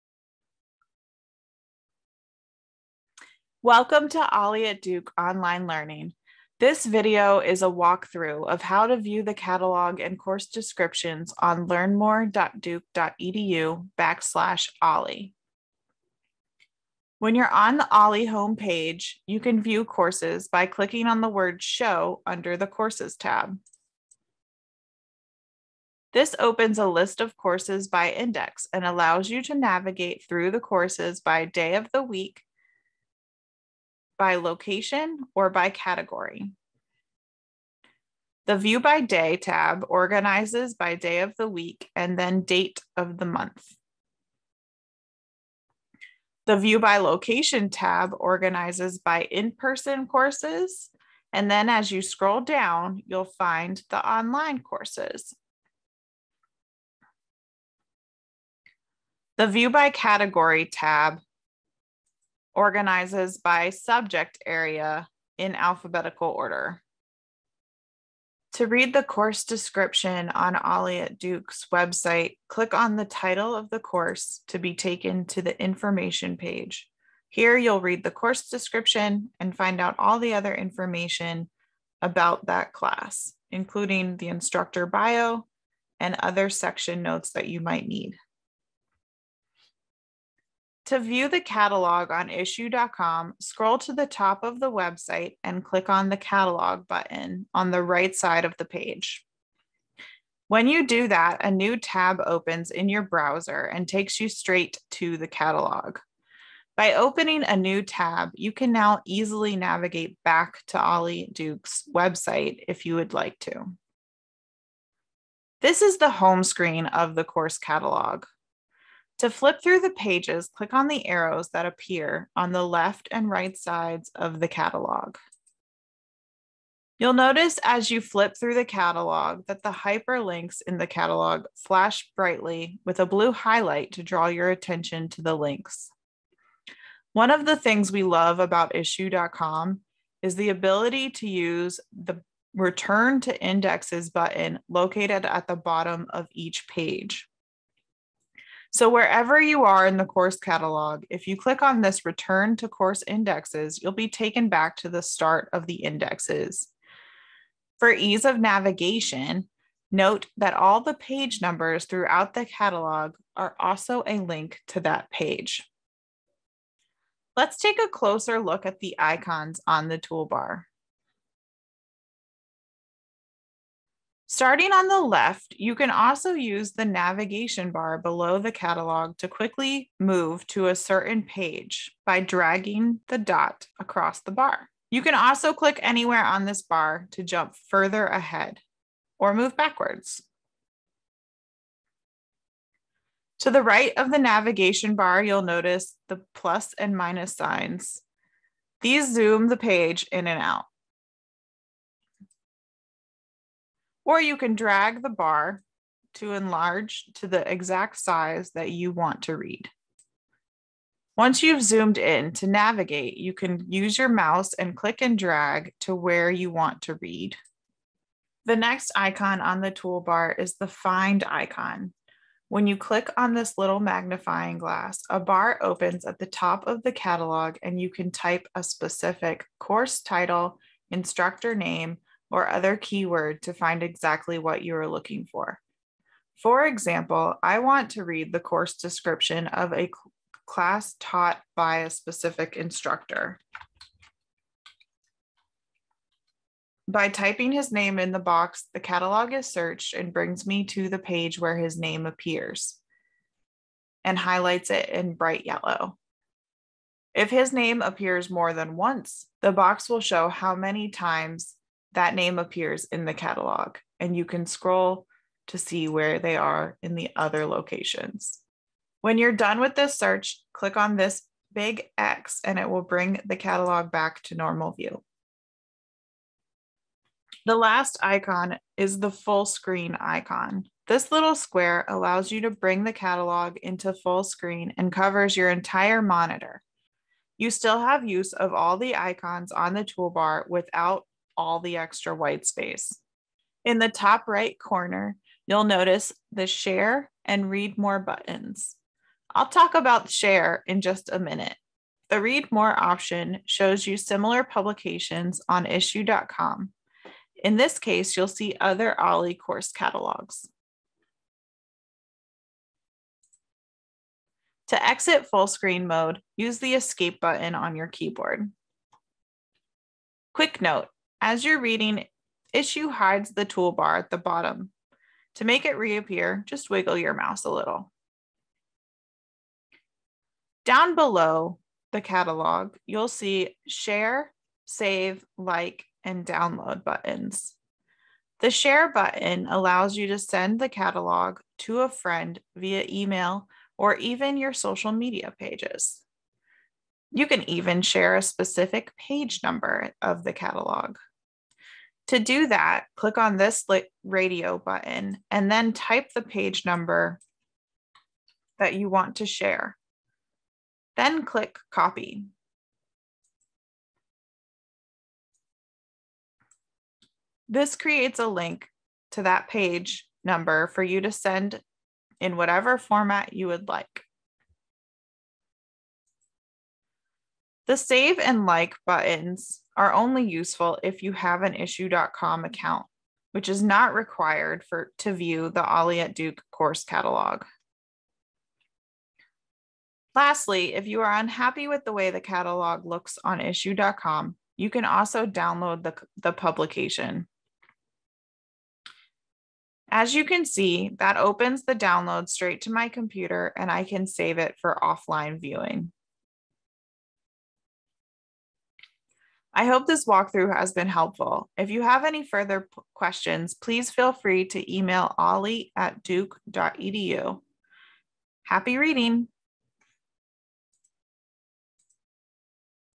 Viewing OLLI at Duke Course Catalog Online
OLLI at Duke Courses and Catalog walkthrough. This video explains how to use to view course information as well as how to navigate the course catalog Click show more to see video bookmarks.

Timecodes
0:00 - Intro
0:15 - Viewing courses on Learnmore.duke.edu/olli
1:30 - View catalog on Issuu.com
2:46 - Using the navigation bar
3:11 - Zooming in and out
3:38 - Find a keyword
4:35 - Fullscreen viewing
5:28 - Sharing the catalog
6:27 - Downloading the catalog